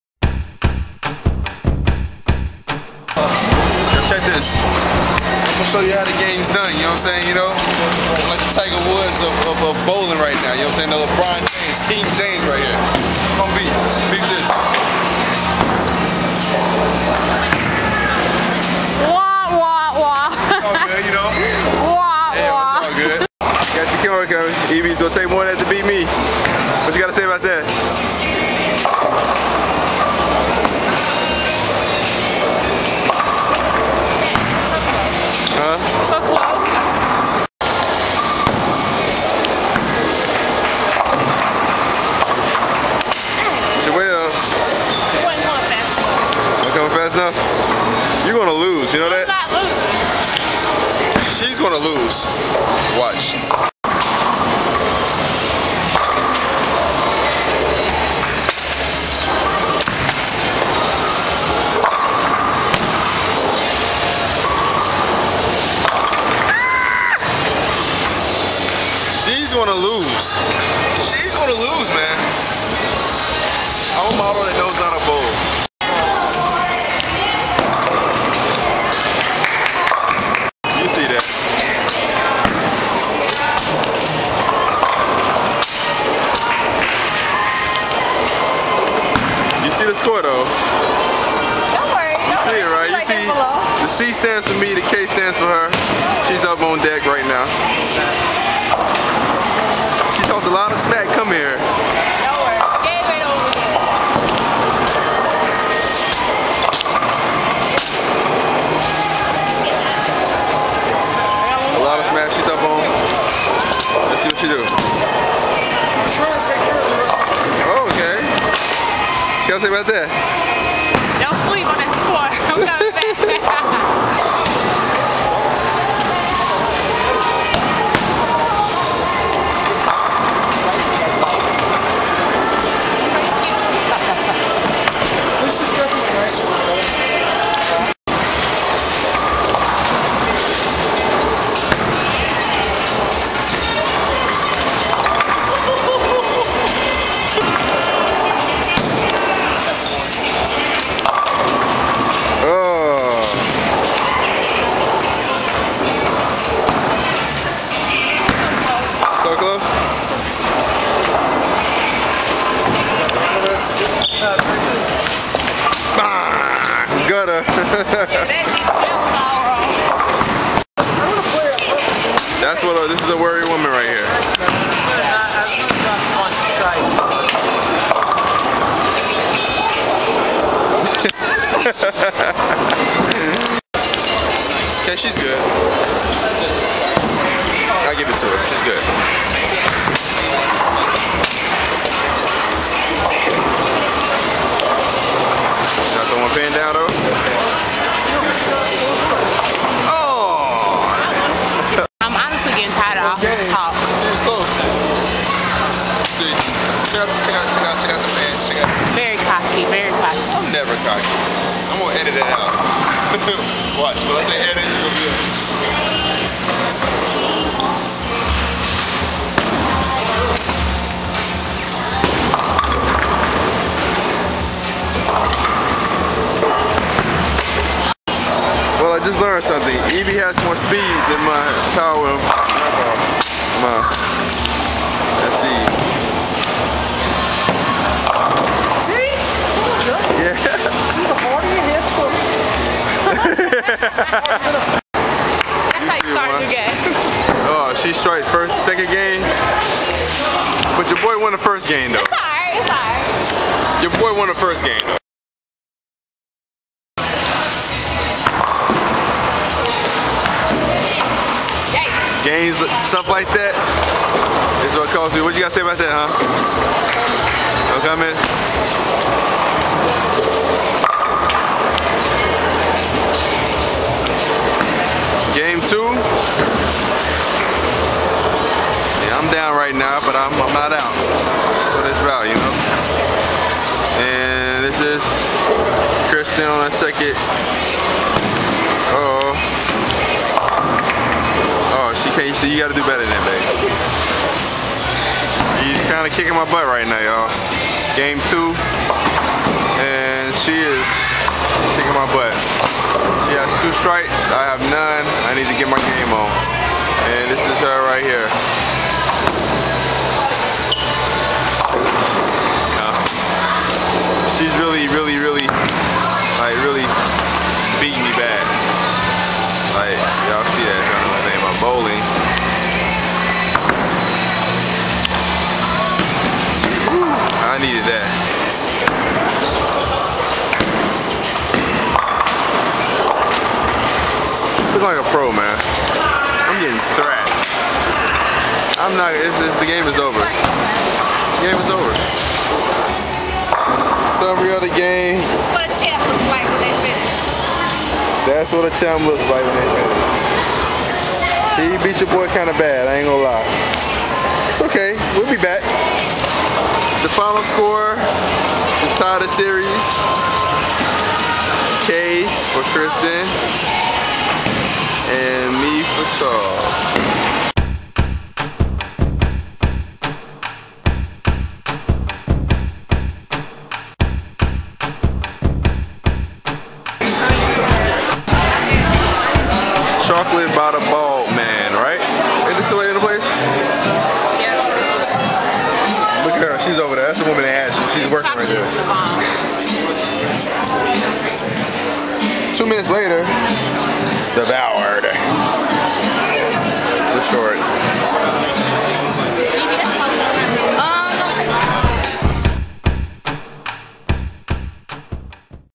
MODELCLUE.COM PRESENTS "BOWLING DAY" A WORK DAY OFF!
NYC Models having fun at 300 bowling lanes at Pier 60 and snacking at Chocolate by the Bald Man in Union Sq.